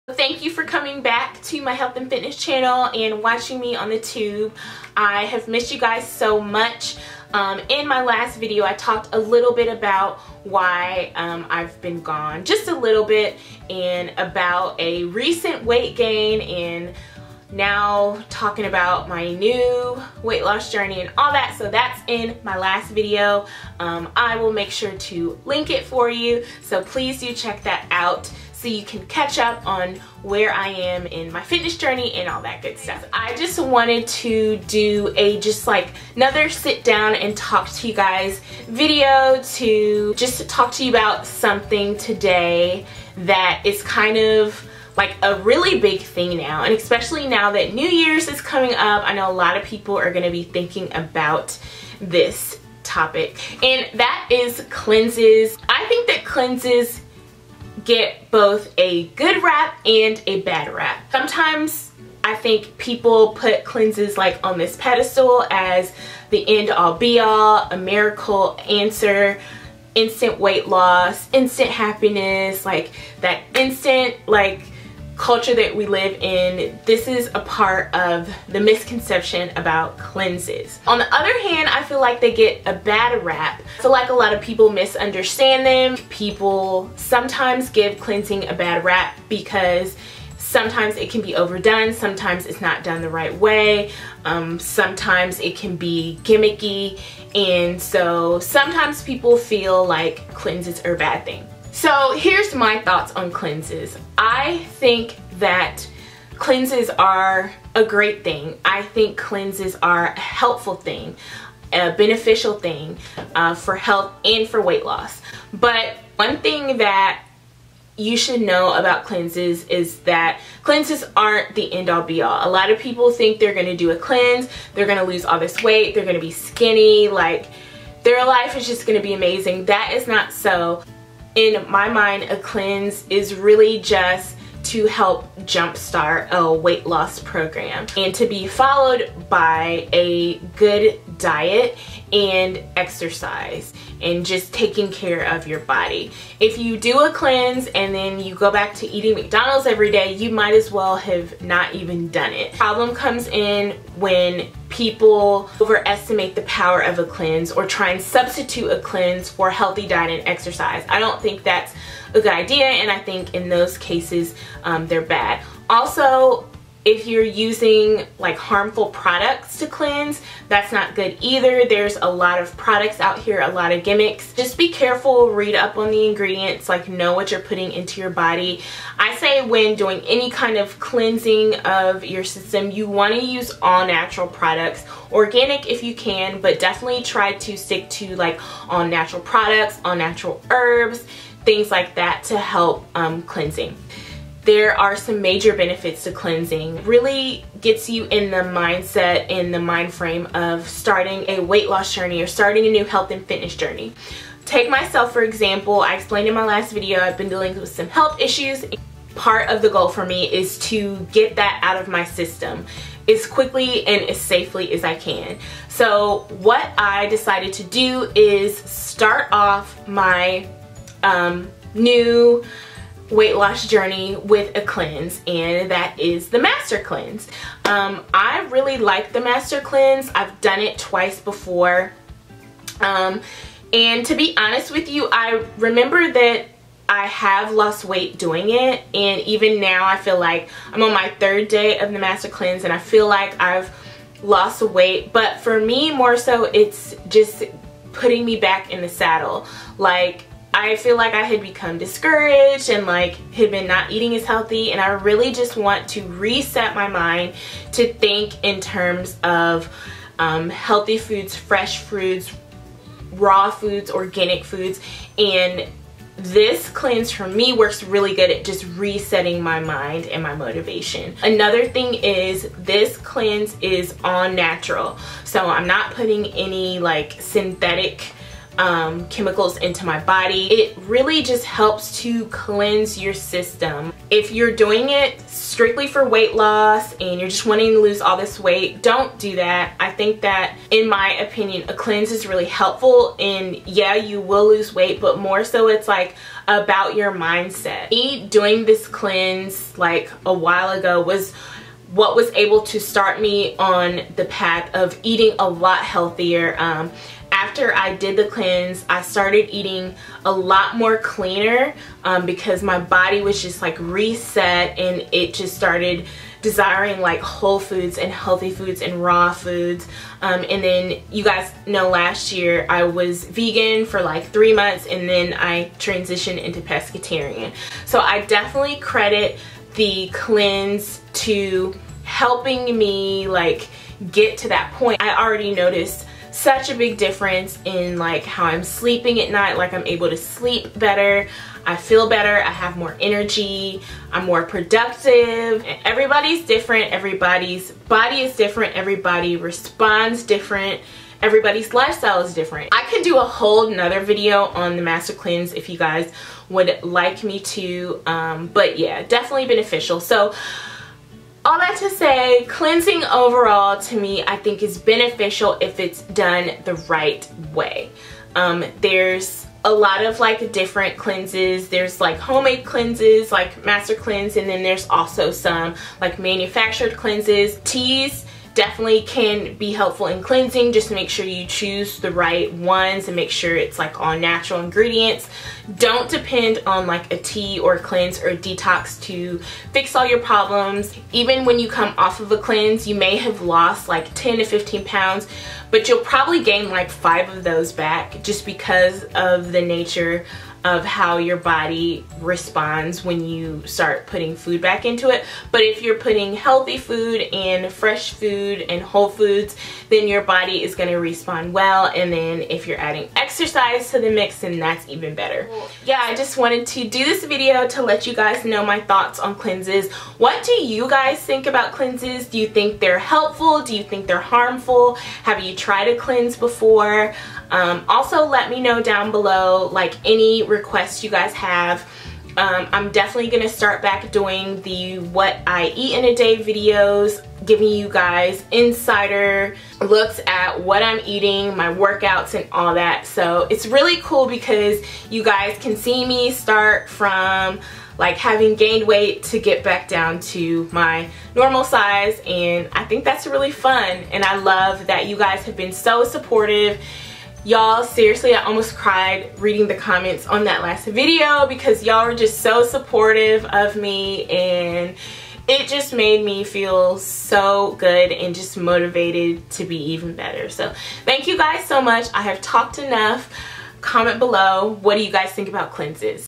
The Truth About Cleanses And Weight Loss
The New Year is approaching and this will be the primary weight loss tactic for many. But it is helpful or harmful? Watch to find out the Truth About Cleansing.

Facebook.com/BodyBeautyBrilliant

I LOVE You and GOD loves you!
xoxoxoxoxo
Ciara